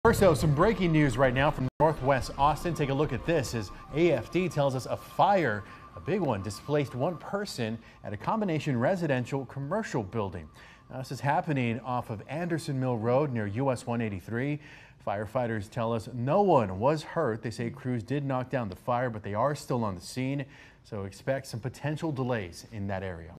AFD say fire ‘knocked down’ after responding to 2-story structure fire
One person has been displaced, and no injuries were reported after the Austin Fire Department said it responded to a two-story structure fire in northwest Austin Wednesday night.